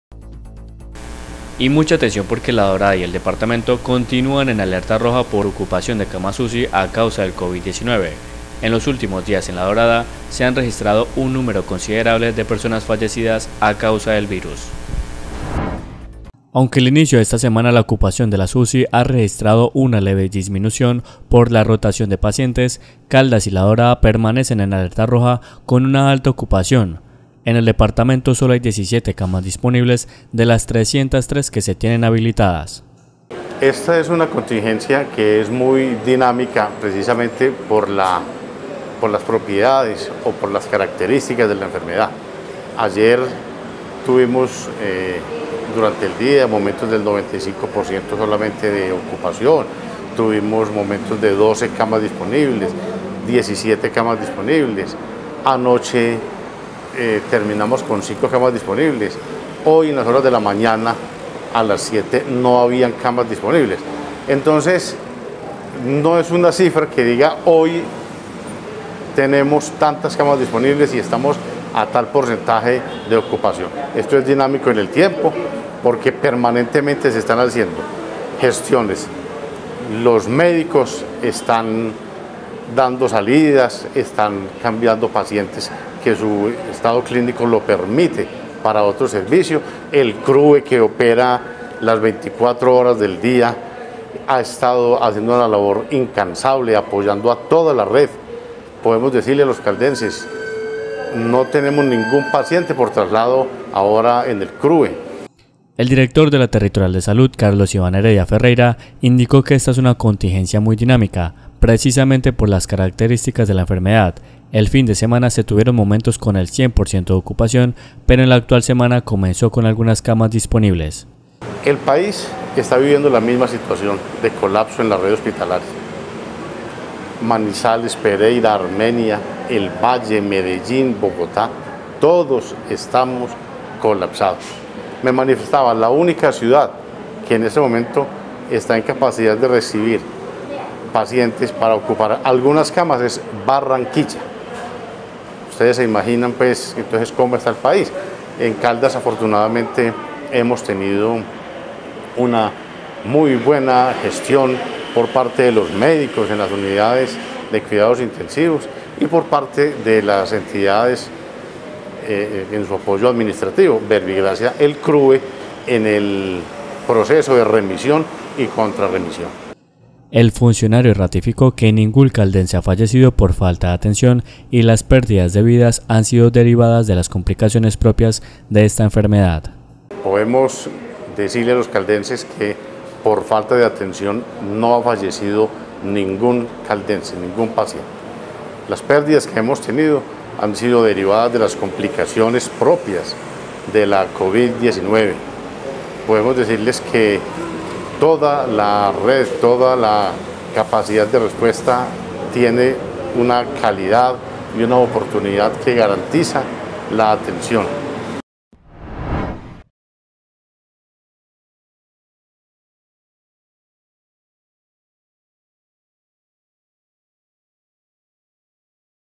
LA DORADA Y EL DEPARTAMENTO CONTINÚAN EN ALERTA ROJA POR OCUPACIÓN UCI
Aunque al inicio de esta semana la ocupación de las UCI ha registrado una leve disminución por la rotación de pacientes, Caldas permanece en alerta roja con una ocupación del 95,3%, con solo 17 camas disponibles, de las 303 que se tienen habilitadas.